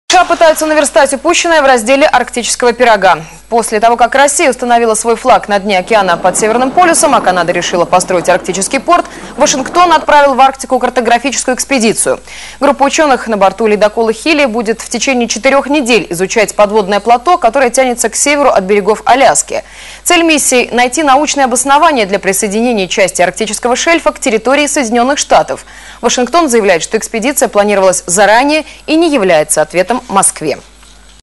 американцы в Арктике 2007
Вашингтон отправил в Арктику картографическую экспедицию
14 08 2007